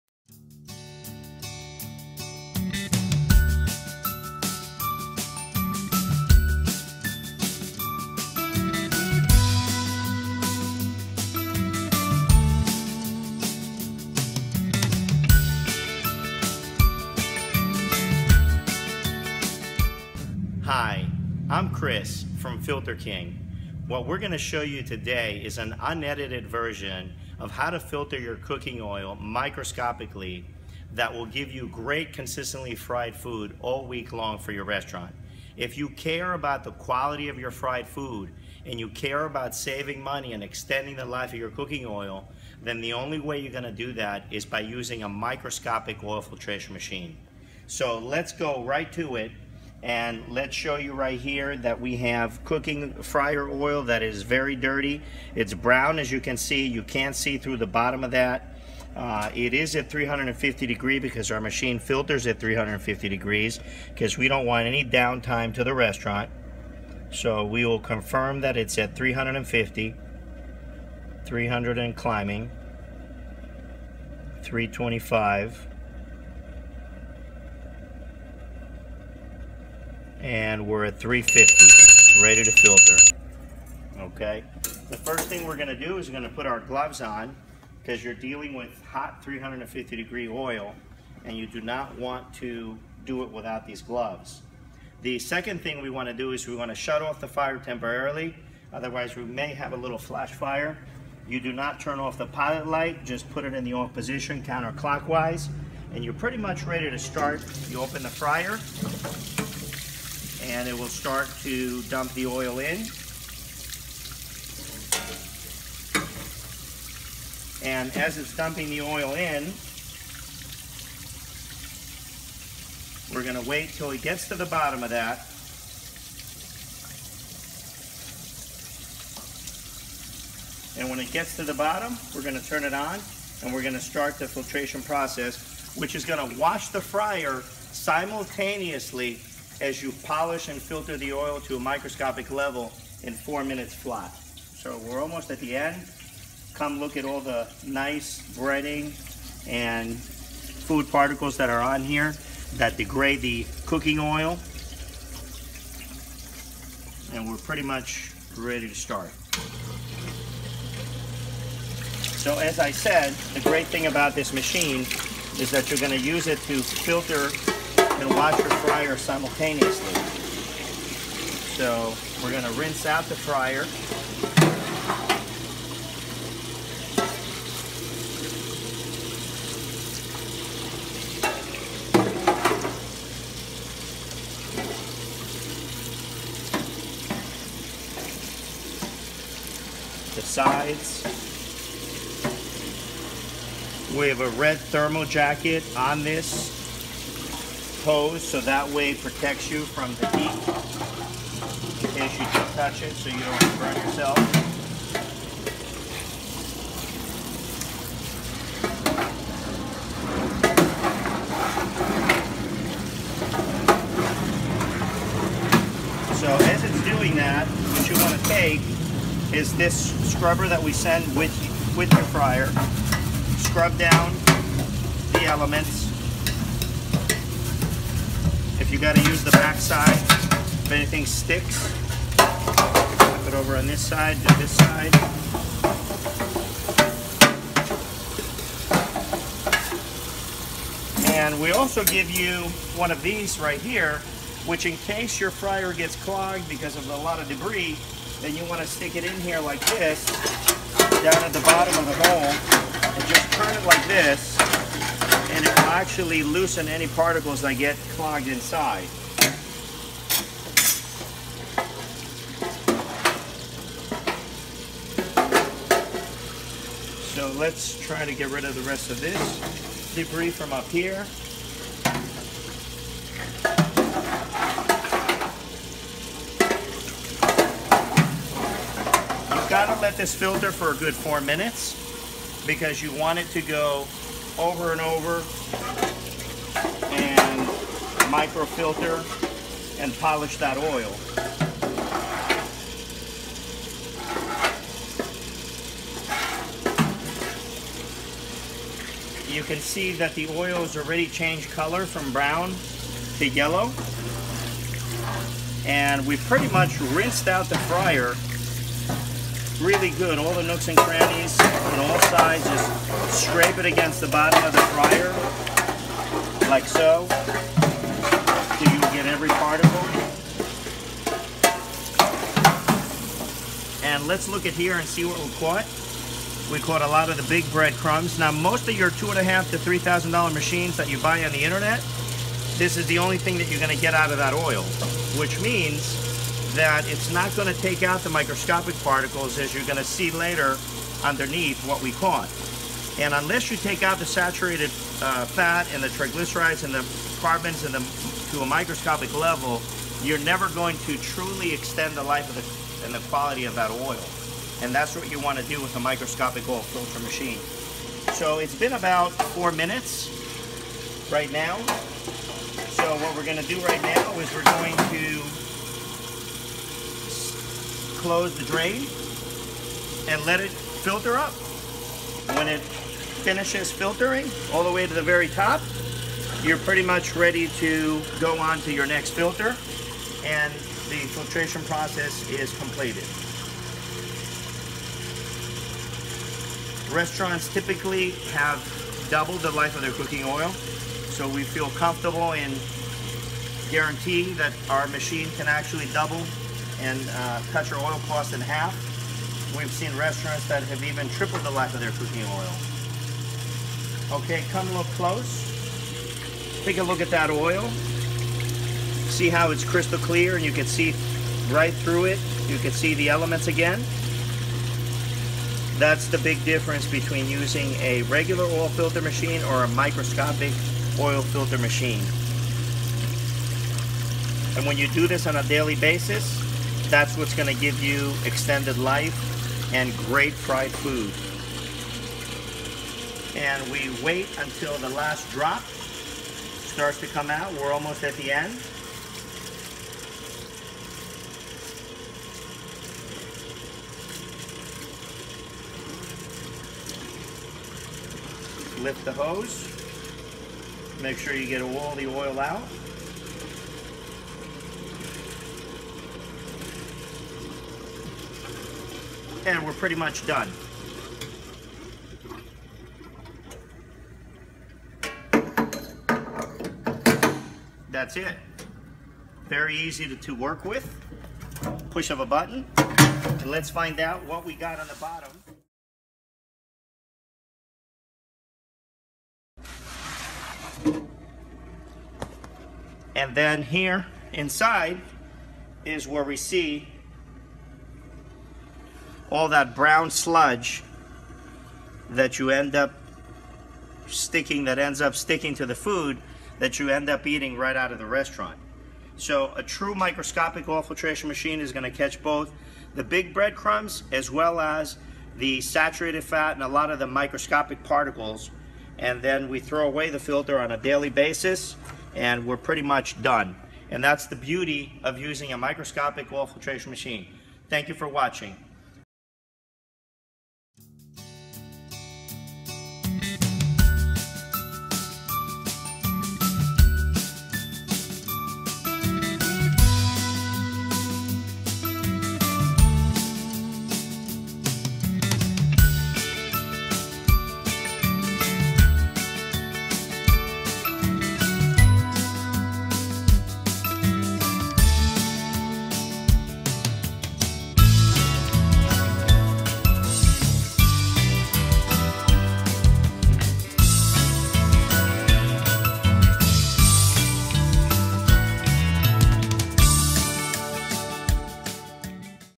Filter King Demonstration
Filter King is heavy duty built for commercial applications with American parts and labor, 1/3 HP motor and 5 Gal per minute reversible pump. The cooking oil passes through 2 unique filter systems which force and seal every drop through a microscopic filter process before returning to the fryer. We call this process the, "Double forced & sealed filtration process" which is the science behind our technology making it vastly different than many other machines in today's market.

Key Features Of Filter King
- Heavy duty, 1/3 HP Haight/Leeson Motor, high heat rated motor for up to 350 degrees for hot oil filtration.
- Two speed Pump 5 gal per minute flow, for fast and deep oil filtration to as low as 1-20 micron using double filter system.
- Forced and double sealed filtration system, forcing every drop of oil to pass through microscopic filter.
- Filter #1: Stainless steel 300 micron screen basket.
- Filter #2: "Forced & double sealed Micro-filter rack" and micro pads that filters down to 1-20 microns, removing microscopic impurities that degrade the oil.
- Total weight of unit: 90 lbs. on 4 wheels, front swivel.
- Machine dimensions: 17" x 24" high x 30" long.
- Two speeds; Forward and Reverse pumping option.
- Collapsible handle for easy storage.
- Stainless Steel 304 food grade construction all around, inside and out for easy cleaning and long lasting.
- Anti-spill locking lid for transporting hot oil to dumpster.
- Four wheels for easy mobility and collapsing handle for tight storage.
- Heat insulated hose up to 500 F degrees for easy handling so it never burns you.
- 16 gallon tank (120 lbs.) to hold oil from big fryers.
- Filters and cleans an average 35lb fryer in 4 minutes & 85lb fryer in 6 minutes.
- 1 Year limited warranty for motor and pump, and total machine.
- Leasing is available to qualified buyers in the USA with 2 years in business and good credit.
- Special pricing for purchase of multiple units
Check out Filter King's Fryer Filter Machine by visiting this